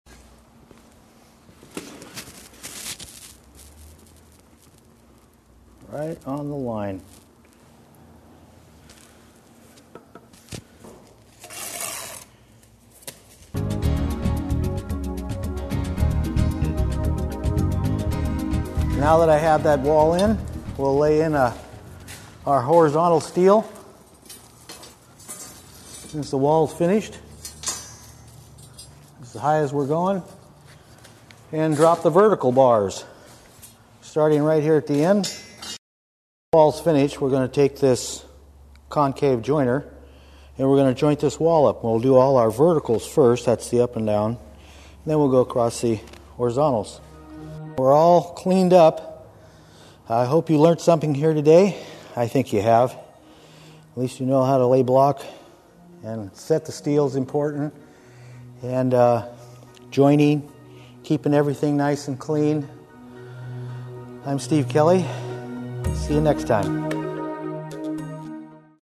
Building a Block Wall
Block walls made simple with this step-by-step DVD.

Steven Kelley provides a series of do it yourself how-to instructional masonry DVDs for working with brick mortar and stone.